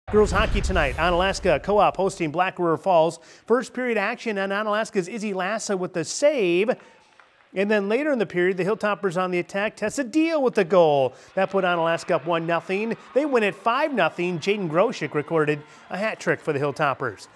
Groshek leads Onalaska hockey over BRF
The Onalaska girls Co-op shutdown Black River Falls 5-0 Thursday night.